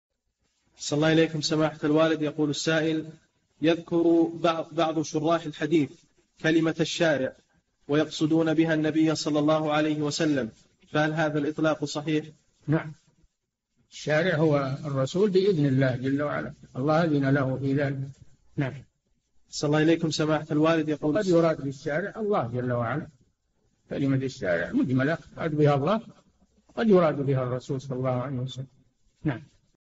[2075- 4717] هل يجوز إطلاق لفظ الشارع ويراد به الرسول صلى الله عليه وسلم؟ - الشيخ صالح الفوزان
[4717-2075] يذكر بعض شراح الحديث كلمة ((الشارع)) ويقصدون بها النبي صلى الله عليه وسلم فهل هذا الإطلاق صحيح؟
الجَوَابُ: نعم، الشارع هو الرسول بإذن الله جل وعلا، الله أذن له في ذلك، وقد يراد بالشارع الله جل وعلا، فكلمة الشارع مجملة قد يراد بها الله وقد يراد بها الرسول صلى الله عليه وسلم.
السيرة الذاتية :
للاشتراك في قناة الشيخ صالح الفوزان
لمشاهدة حلقات فتاوى على الهواء كاملة

للاستفسار والإقتراحات والدعم
رئيس الفريق
لمشروع كبار العلماء